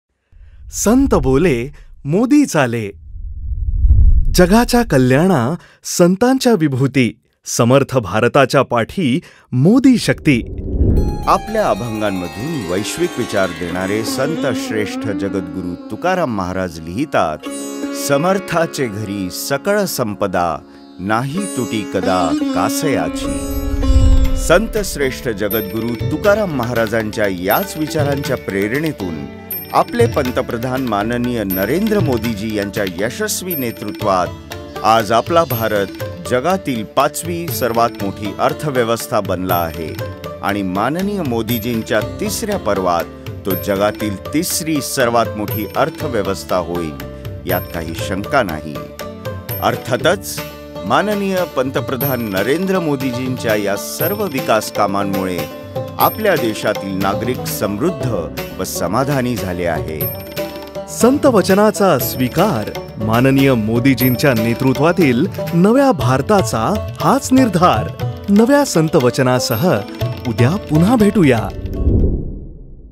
|| संत बोले मोदी चाले || मा.मोदीजींच्या नेतृत्वात भारत जगात तिसऱ्या क्रमांकाची अर्थव्यवस्था होणार !
|| संत बोले मोदी चाले ||
मा.मोदीजींच्या नेतृत्वात भारत जगात तिसऱ्या क्रमांकाची अर्थव्यवस्था होणार !